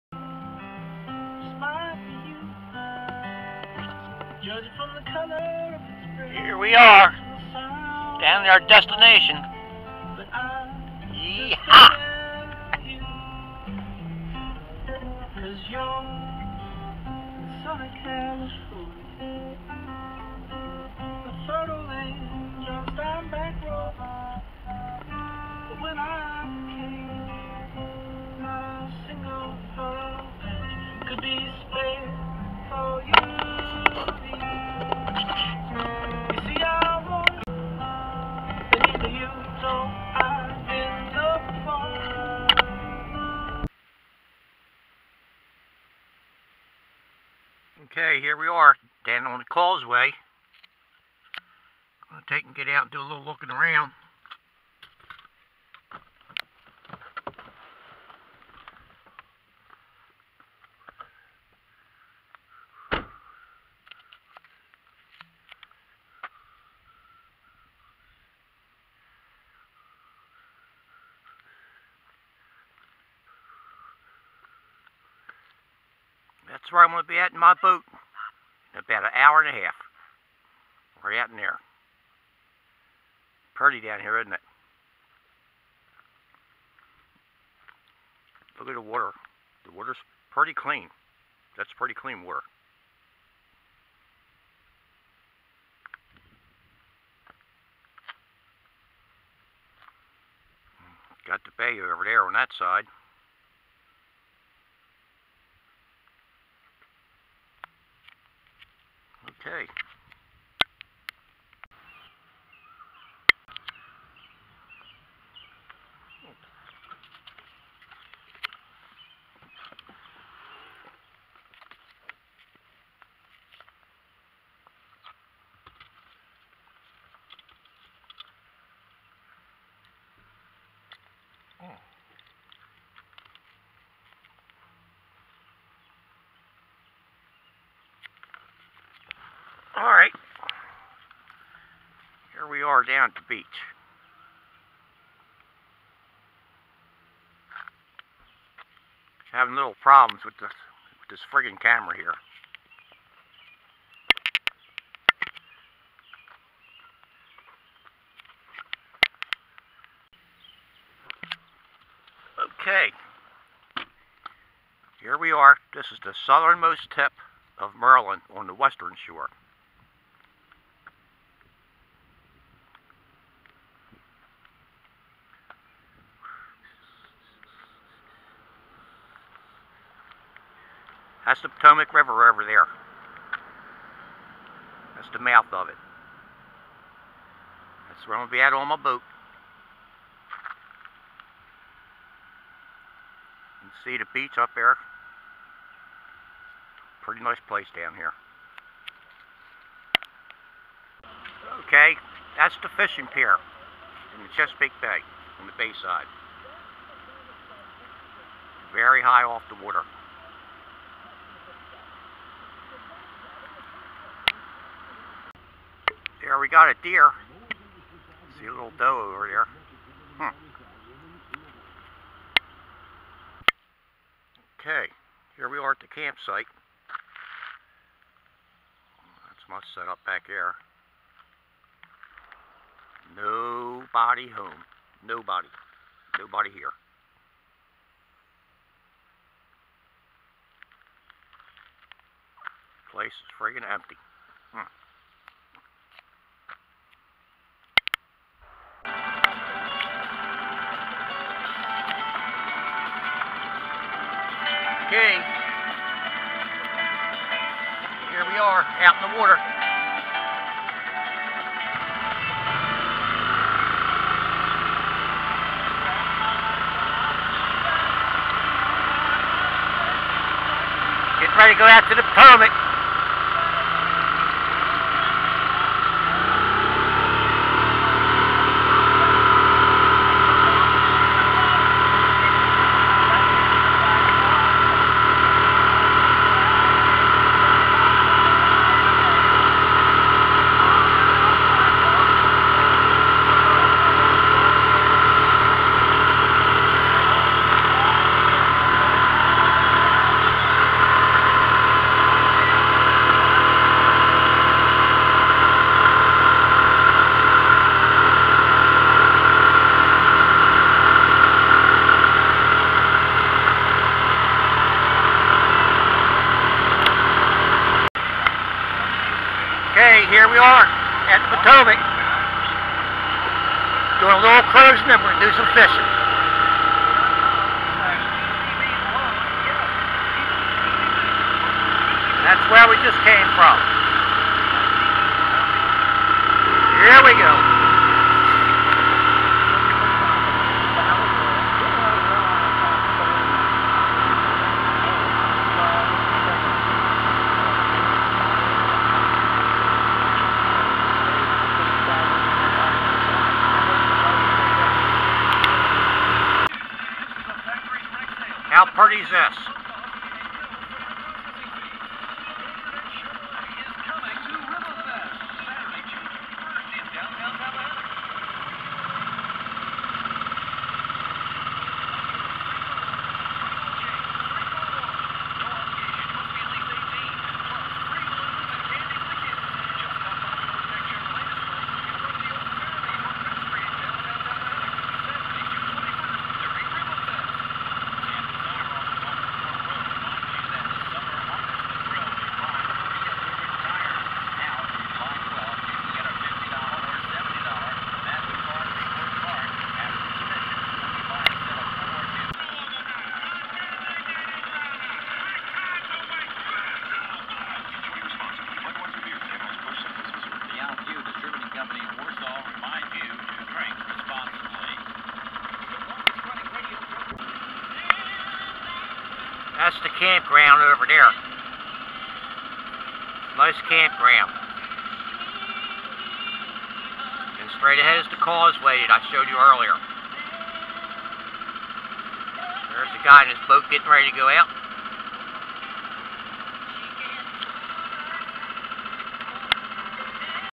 point lookout state park md.
my camping trip to point lookout nice place to camp and fish